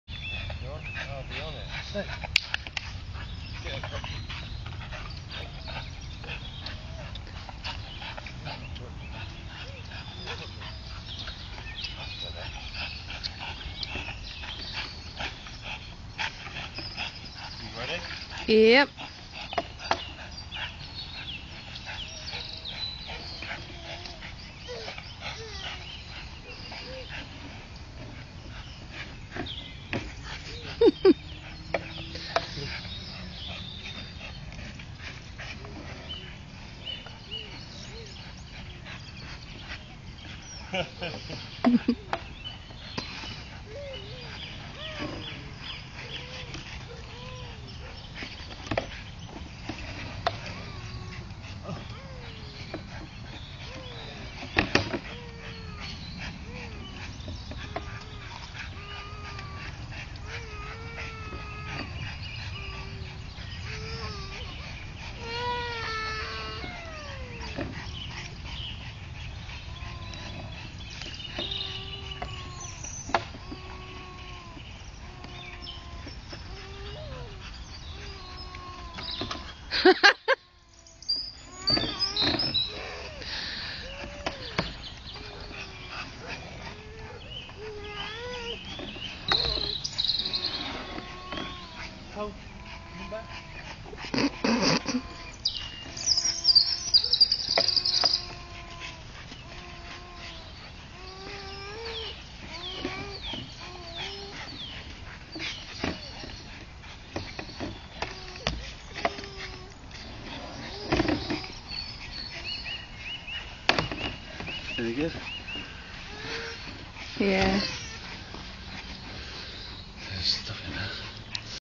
Crazy Staffy
Staffy on rope swing